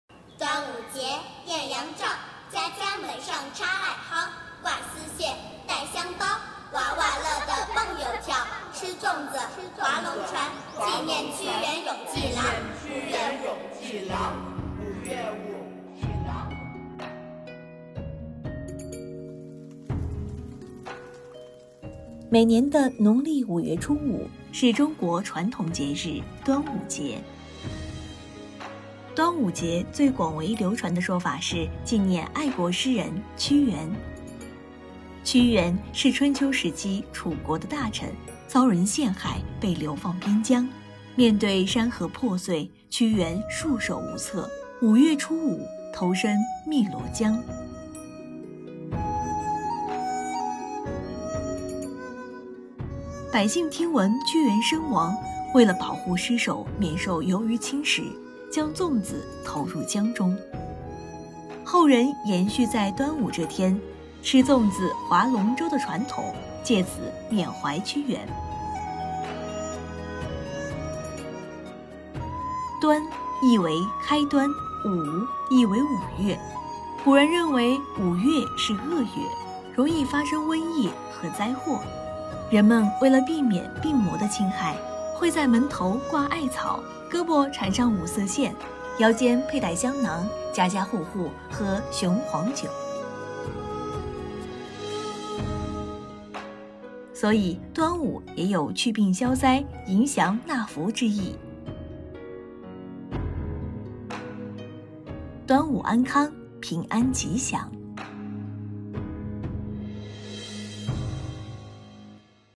一年一端午，一岁一安康 The annual Dragon Boat Festival, for well-being and healthy
一个个香甜软糯的粽子，是传统文化的世代延续，一株株清香弥漫的艾草，是招福驱邪的美好愿景。

Sweet and soft zongzi are the continuation of traditional culture from generation to generation. The fragrant wormwood leaves are the good vision of blessing and warding off diseases and disasters.